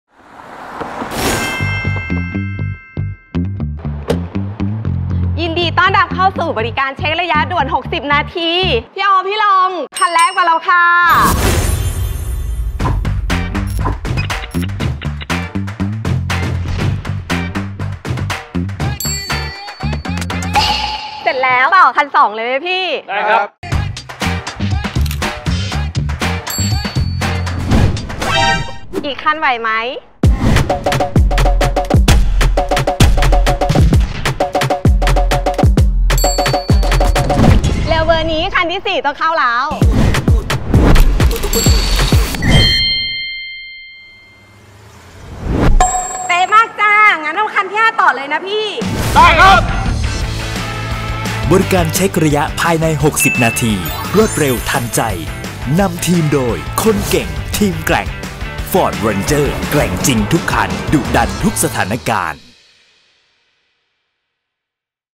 ฟอร์ดคนเก่งทีมแกร่ง บริการเช็กระยะภายใน 60 นาที จากฟอร์ด รวดเร็ว ทันใจ!
ตามติดภารกิจ 1 วันกับช่างผู้เชี่ยวชาญของฟอร์ด!
กับบริการตรวจเช็กระยะภายใน 60 นาที! รวดเร็ว ทันใจ 🕐 👍
ทีมงานคนเก่ง ทีมแกร่งพร้อมบริการเต็มที่ทุกคัน! จองรับบริการง่ายๆ ผ่านแอปฯ FordPass
รายละเอียดเพิ่มเติมคลิก
นำทีมโดย